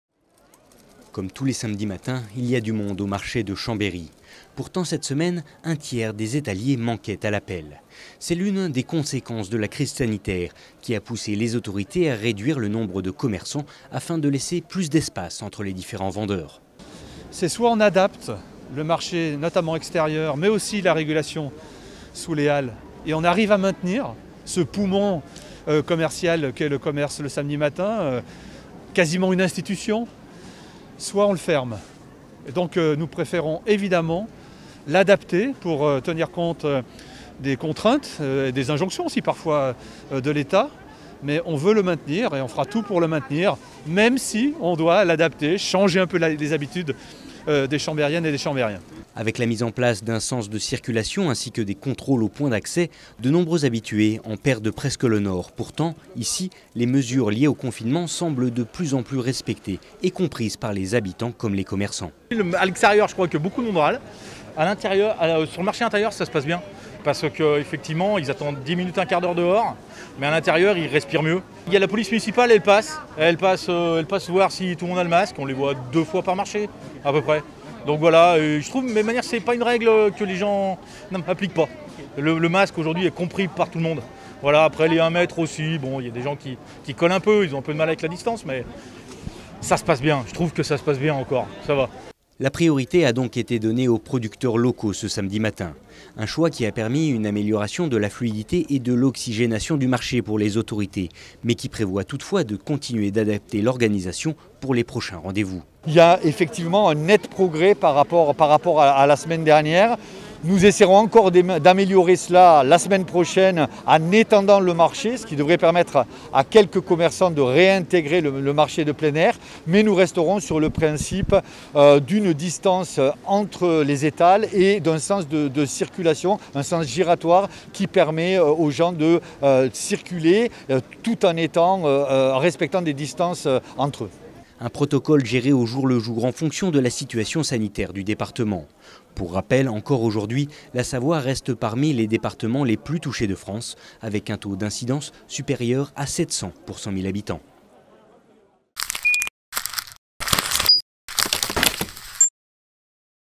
Un marché adapté à Chambéry
Si à ce jour le département de Savoie n’est plus en tête des territoires les plus touchés par la covid 19, les autorités chambériennes ont toutefois décidé d’adapter l’organisation de leurs marchés. Moins d’étals pour plus d’espace, une mesure sera amenée à évoluer en fonction de la situation sanitaire. Ce weekend, Emmanuel Jaud s’est rendu dans la Cité des Ducs, où ces nouvelles mesures étaient suivies de près par les autorités.